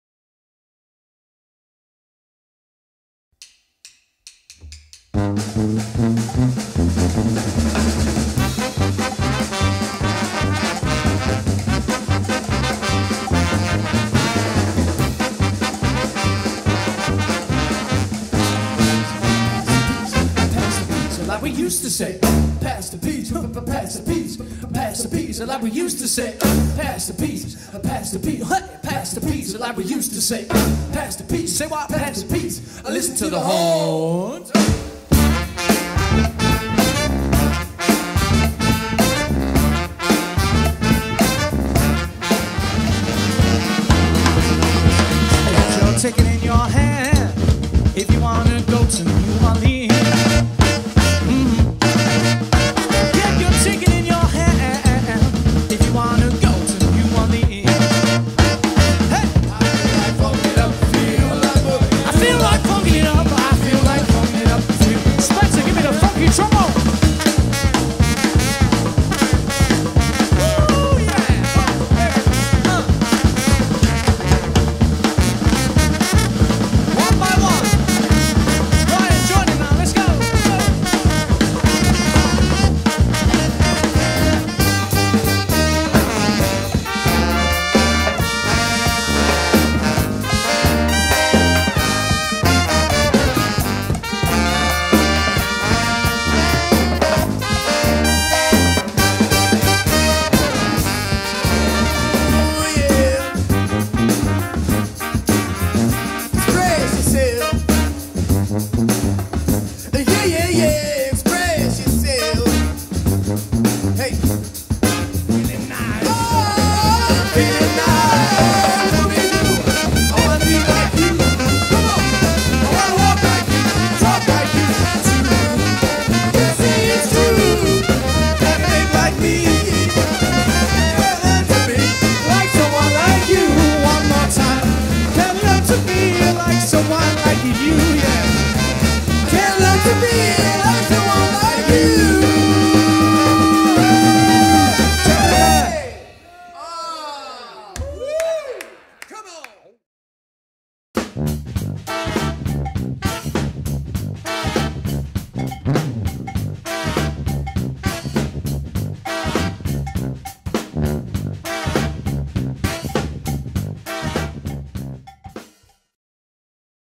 King Brasstards Live During Lockdown Promotion Video
During Covid Lockdown 2020, King Brasstards have recorded a series of live videos. This is a taste of what's to come